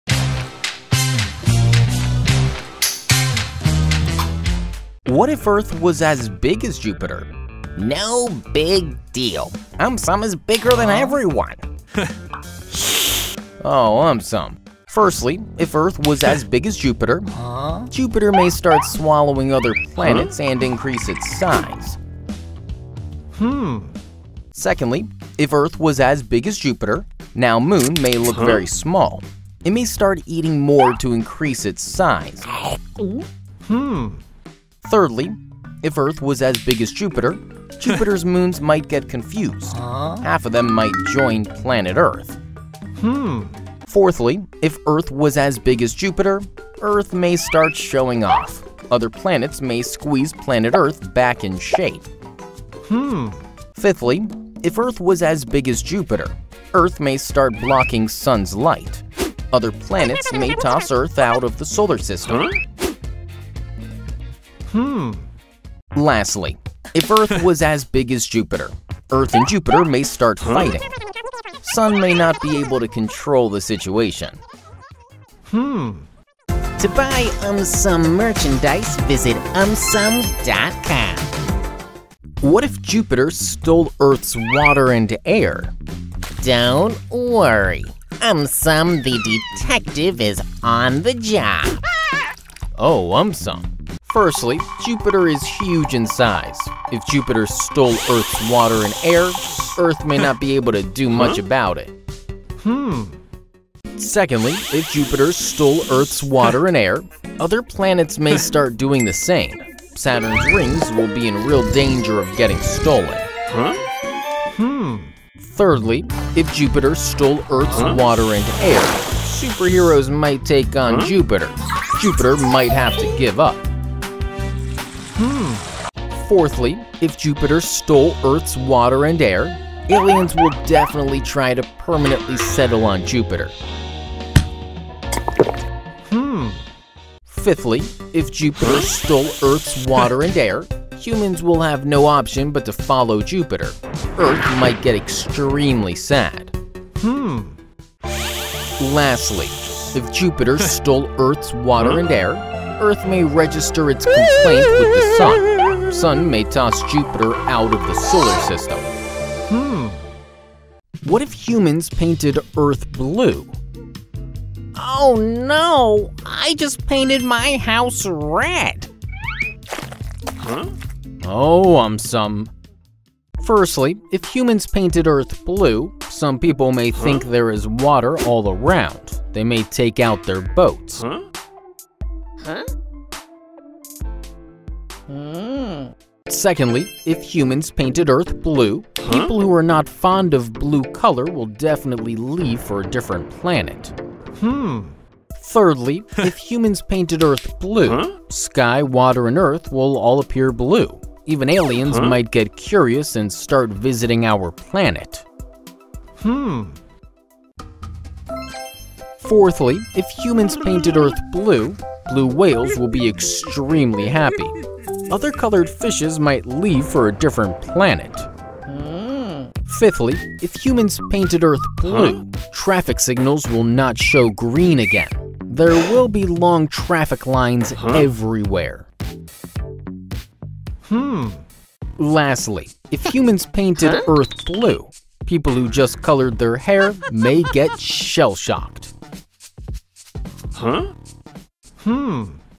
What if Earth was as Big as Jupiter? + more videos
What if Earth was as Big as Jupiter?
Firstly, if Earth was as Big as Jupiter, Jupiter may start swallowing other planets and increase its size.
Secondly, if Earth was as Big as Jupiter, now Moon may look very small, it may start eating more to increase its size.
Thirdly, if Earth was as Big as Jupiter, Jupiter's Moons might get confused, half of them might join planet Earth.
Fourthly, if Earth was as Big as Jupiter, Earth may start showing off, other planets may squeeze planet Earth back in shape.
Fifthly, if Earth was as Big as Jupiter, Earth may start blocking Sun's light, other planets may toss Earth out of the Solar System.
Lastly, if Earth was as Big as Jupiter, Earth and Jupiter may start fighting, Sun may not be able to control the situation.

Timecodes
0:00 - What if Earth was as Big as Jupiter?
1:22 - What if Jupiter Stole Earth's Water and Air?
2:40 - What if Humans painted Earth Blue?